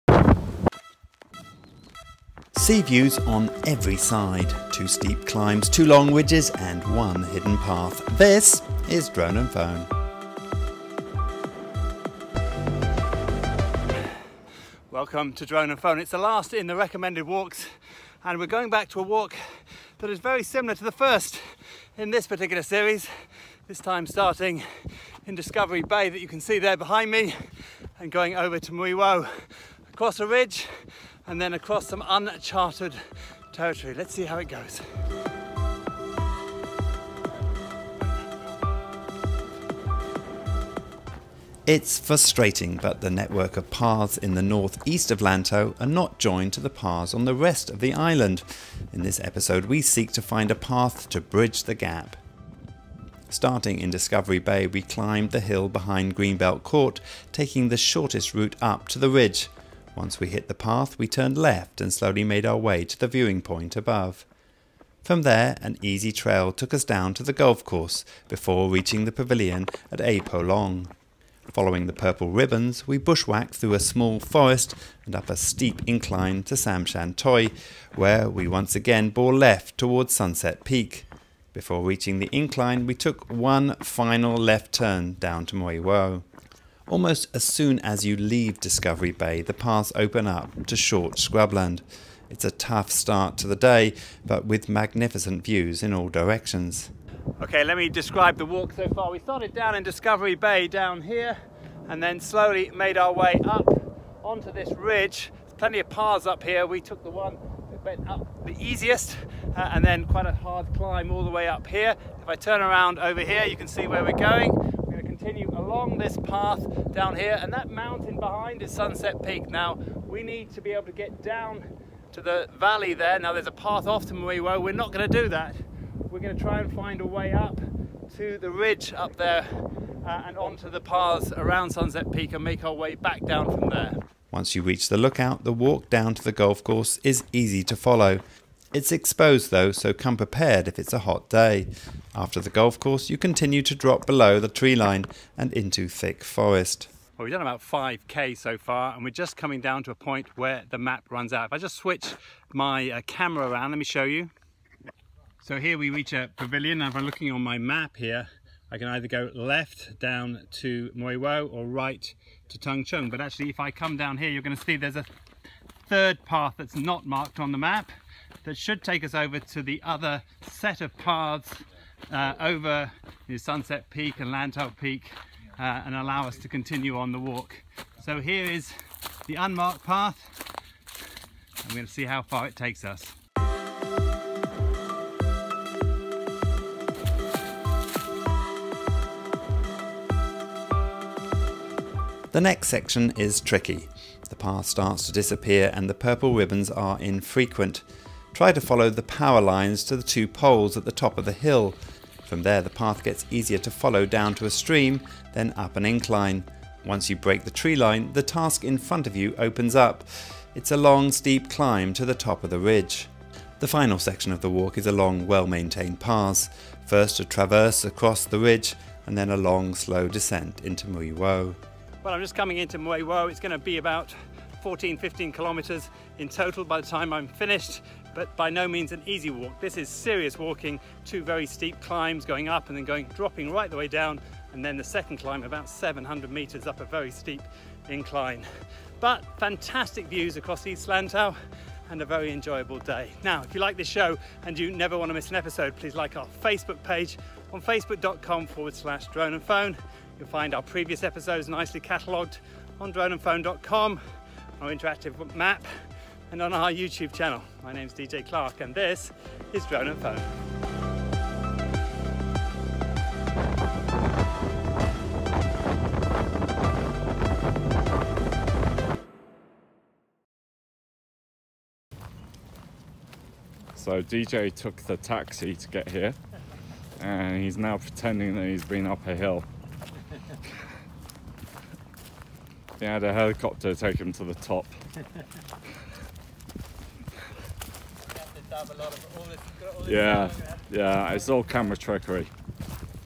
Discovery Bay to Mui Wo | RECOMMENDED WALKS
In the final walk recommended by our viewers, we return to Discovery Bay. This time to take a cruel 16km hike over two ridges in a half circle that brings us back to the coast at Mui Wo.